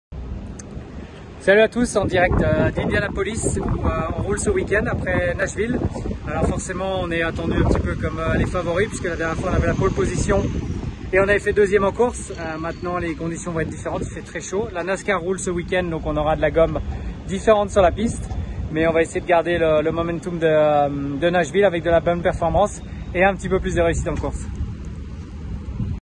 Romain Grosjean : "Garder le momentum"
Romain Grosjean évoque la douzième épreuve de la saison en NTT INDYCAR Series, sur le circuit routier de l'Indianapolis Motor Speedway.